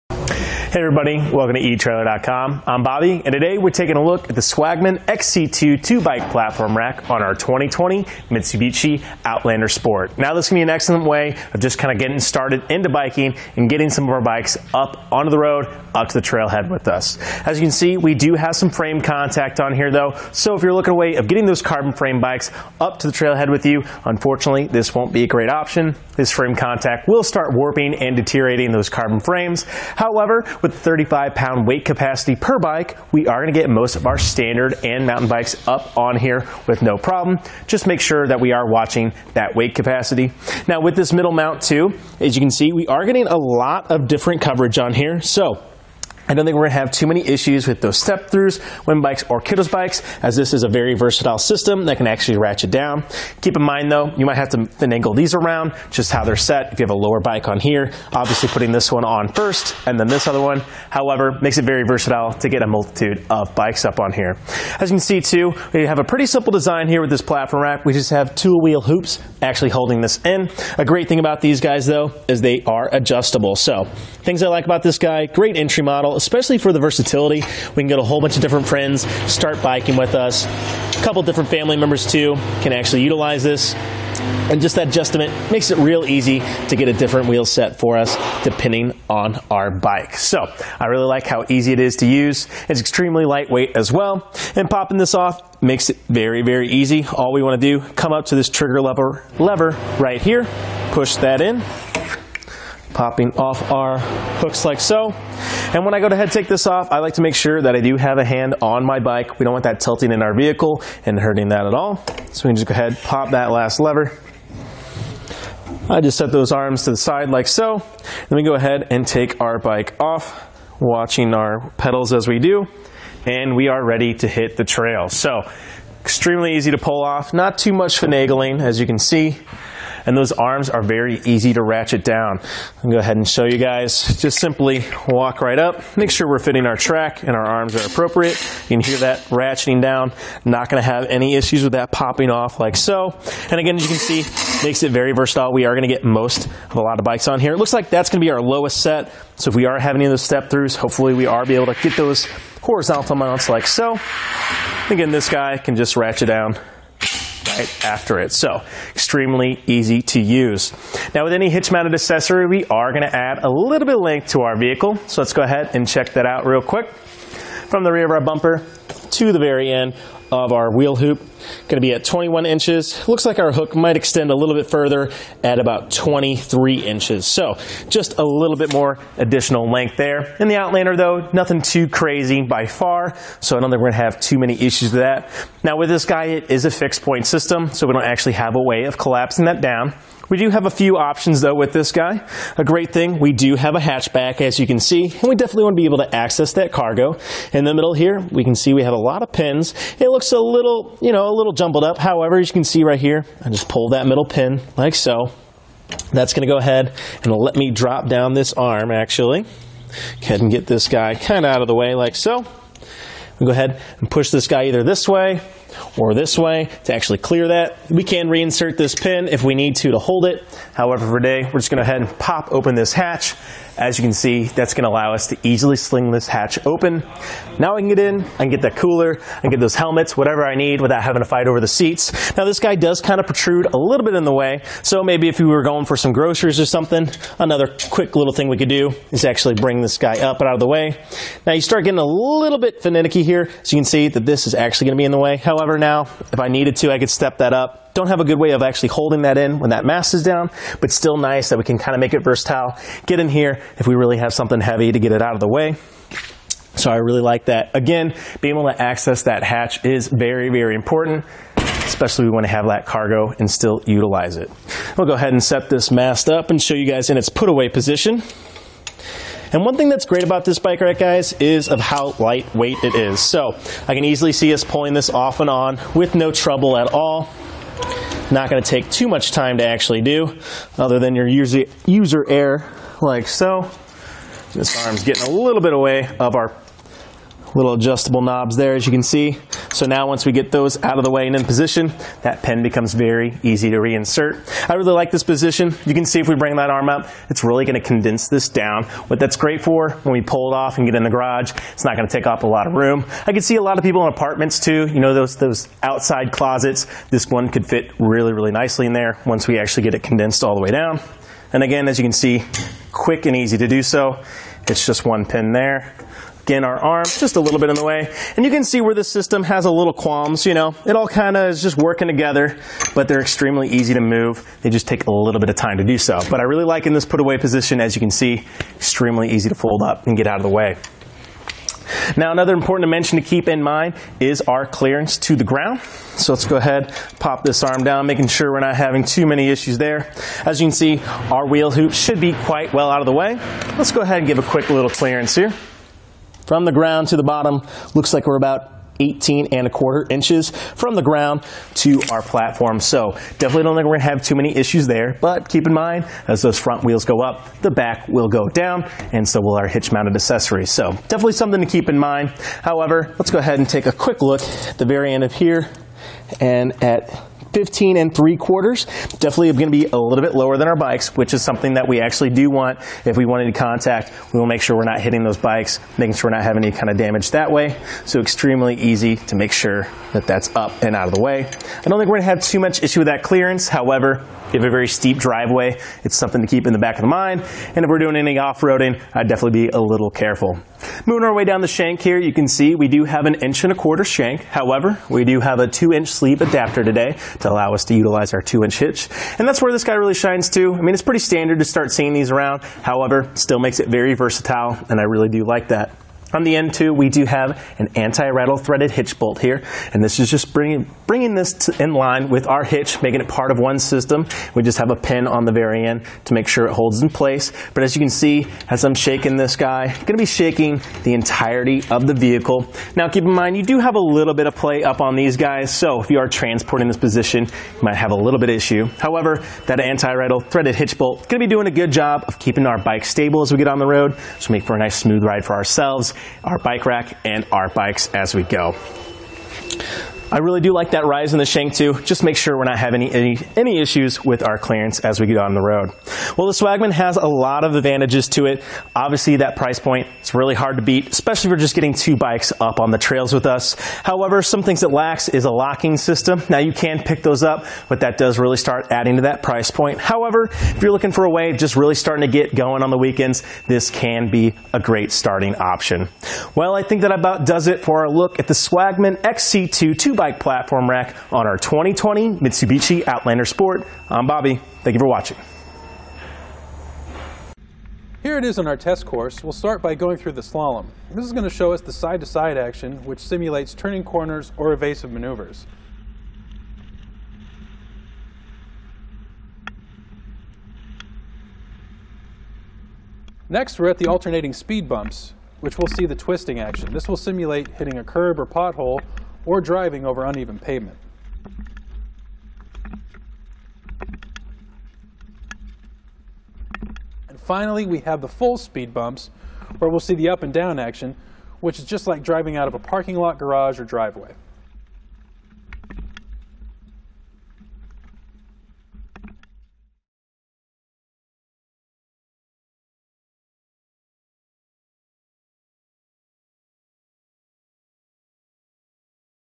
Is the Swagman XC2 Bike Rack for 2 bikes Compatible with the 2020 Mitsubishi Outlander Sport?
Hey everybody. Welcome to etrailer.com. I'm Bobby. And today we're taking a look at the Swagman XC2 two bike platform rack on our 2020 Mitsubishi Outlander sport. Now, this can be an excellent way of just kind of getting started into biking and getting some of our bikes up onto the road out to the trail head with us. As you can see we do have some frame contact on here though.

So if you're looking a way of getting those carbon frame bikes up to the trail head with you, unfortunately, this won't be a great option. This frame contact will start warping and deteriorating those carbon frames. However, with 35 pound weight capacity per bike we are gonna get most of our standard and mountain bikes up on here with no problem. Just make sure that we are watching that weight capacity. Now with this middle mount too, as you can see we are getting a lot of different coverage on here.

So I don't think we're gonna have too many issues with those step throughs, women's bikes or kiddo's bikes as this is a very versatile system that can actually ratchet down. Keep in mind though, you might have to finagle these around adjust how they're set. If you have a lower bike on here, obviously putting this one on first and then this other one. However, it makes it very versatile to get a multitude of bikes up on here. As you can see, too, that you have a pretty simple design here with this platform rack.

We just have two wheel hoops actually holding this end. A great thing about these guys though, is they are adjustable. So things I like about this guy, great entry model especially for the versatility. We can get a whole bunch of different friends start biking with us. A couple of different family members too can actually utilize this.

And just that adjustment makes it real easy to get a different wheel set for us depending on our bikes. So I really like how easy it is to use. It's extremely lightweight as well and popping this off makes it very, very easy. All we want to do come up to this trigger lever right here, push that in, popping off our hooks, like so. And when I go ahead and take this off, I like to make sure that I do have a hand on my bike. We don't want that tilting in our vehicle and hurting that at all. So we just go ahead pop that last lever. I just set those arms to the side, like so. Then we go ahead and take our bike off. Watching our pedals as we do, and we are ready to hit the trail. So extremely easy to pull off. Not too much finagling as you can see. And those arms are very easy to ratchet down. I'll go ahead and show you guys, just simply walk right up make sure we're fitting our track and our arms are appropriate. You can hear that ratcheting down, not gonna have any issues with that popping off, like so. And again, as you can see, makes it very versatile. We are gonna get most of a lot of bikes on here. It looks like that's going to be our lowest set. So if we are having any if those step throughs, hopefully we are be able to keep those horizontal mounts, like so. Again, this guy can just ratchet down right after itself. Extremely easy to use. Now with any hitch mounted accessory, we are gonna add a little bit of length to our vehicle. So let's go ahead and check that out real quick. From the rear of our bumper to the very end of our wheel hoop, gonna be at 21 inches. It looks like our hook might extend a little bit further at about 23 inches. So just a little bit more additional length there In the Outlander though, nothing too crazy by far. So I don't think we're gonna have too many issues with that. Now with this guy, it is a fixed point system. So we don't actually have a way of collapsing that down. We do have a few options though with this guy, A great thing, we do have a hatchback, as you can see. And we definitely wanna be able to access that cargo. In the middle here, we can see we have a lot of pins. It looks a little, you know, a little jumbled up however you can see right here, I just pull that middle pin, like so. That's gonna go ahead and let me drop down this arm, actually. I can get this guy kinda out of the way, like so. we go ahead and push this guy either this way, or this way to actually clear that. We can re insert this pin if we need to, to hold it. However, today we're just going ahead and pop open this hatch. As you can see, that's gonna allow us to easily sling this ha